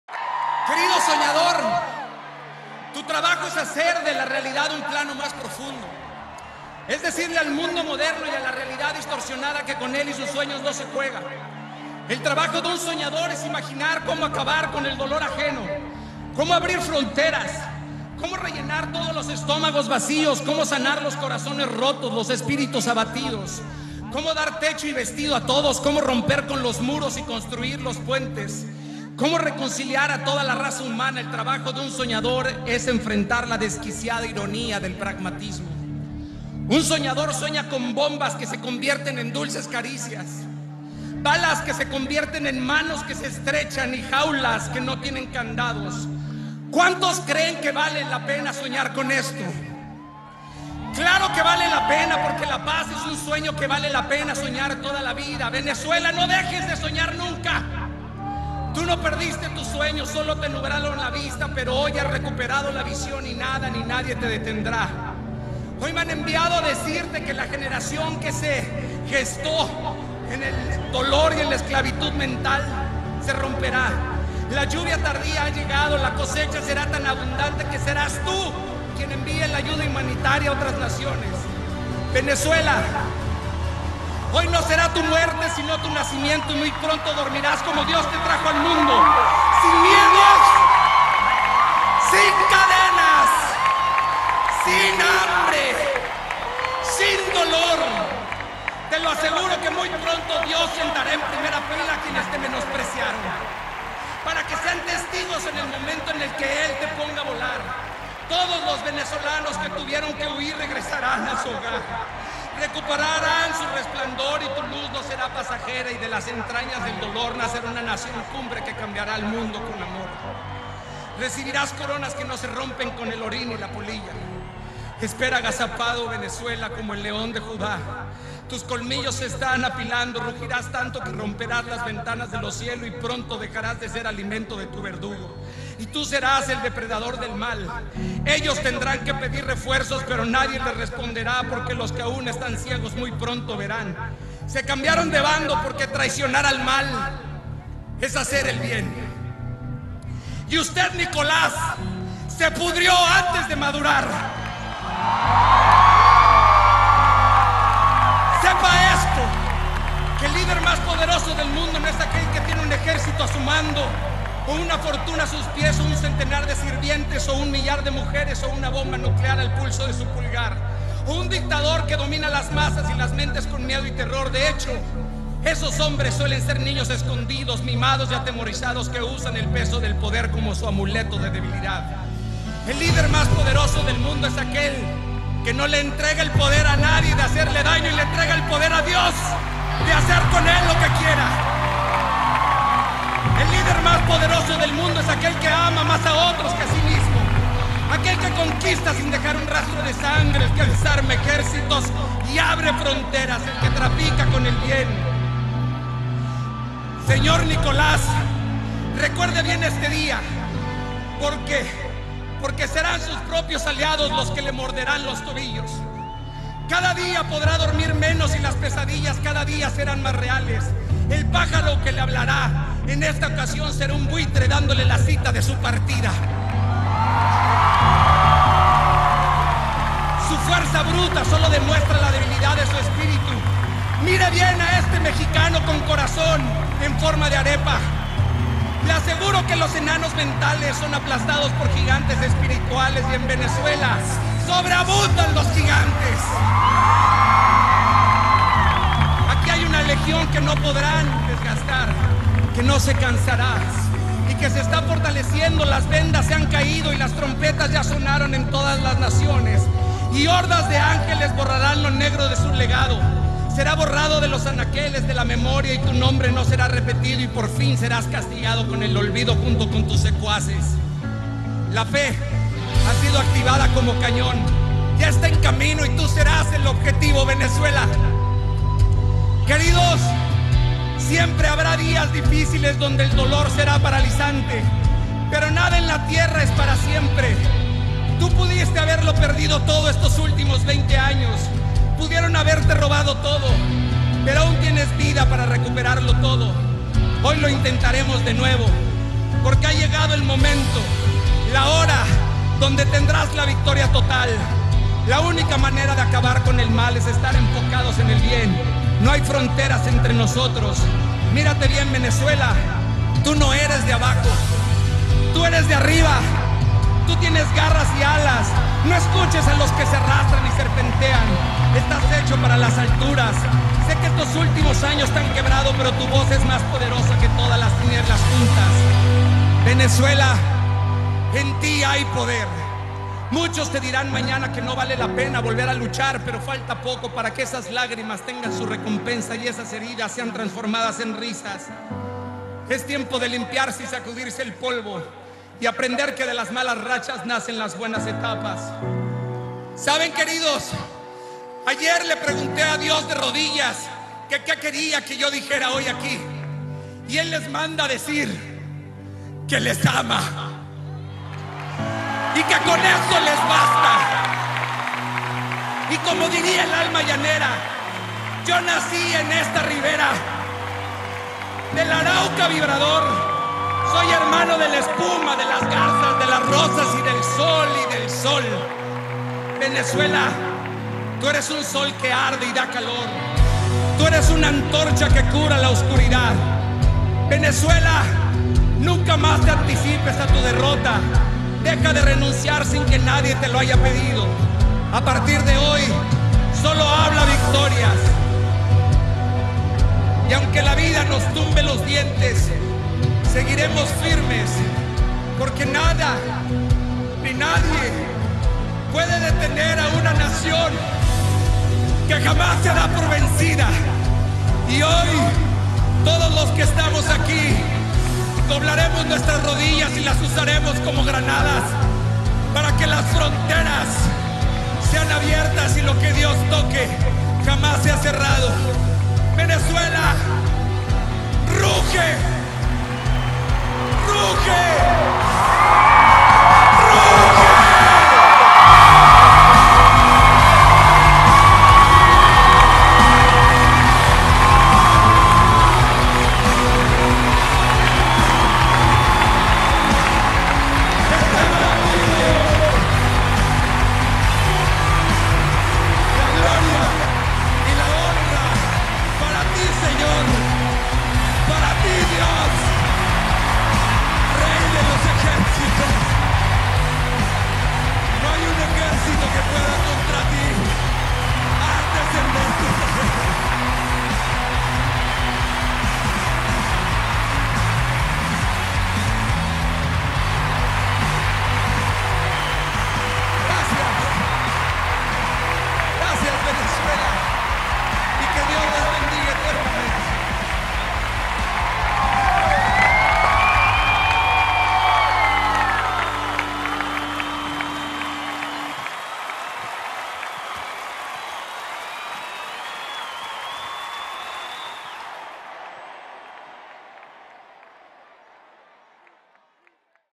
Venezuela RUGE! - Daniel Habif -Traicionar al mal es hacer el bien!
Mensaje de Daniel Habif  Para Venezuela.
Venezuela RUGE! Usted nicolas se pudrió antes de madurar!
(Audio y Video Mejorados)